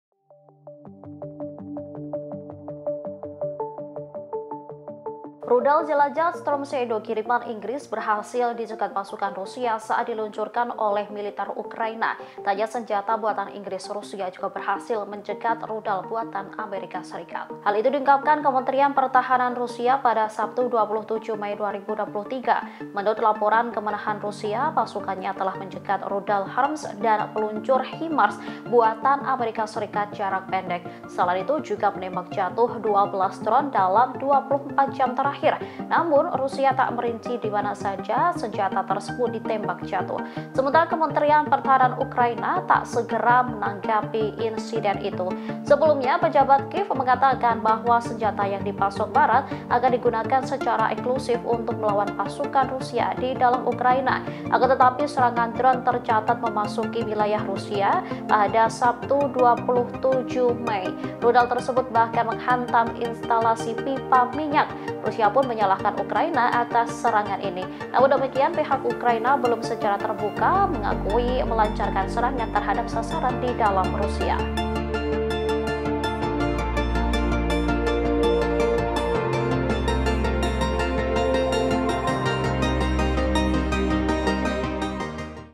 DIGADANG MAMPU MUSNAHKAN RUSIA RUDAL STORM SHADOW INGGRIS HANCUR DI TANGAN MOSKOW
Rudal jelajah Storm Shadow kiriman Inggris berhasil dicegat pasukan Rusia saat diluncurkan oleh militer Ukraina.

Tak hanya senjata buatan Inggris, Rusia juga berhasil mencegat rudal buatan Amerika Serikat.

Hal itu diungkapkan Kementerian Pertahanan Rusia, Sabtu (27/5/2023).

Narator :
Editor : Bramasto Adhy
(fem)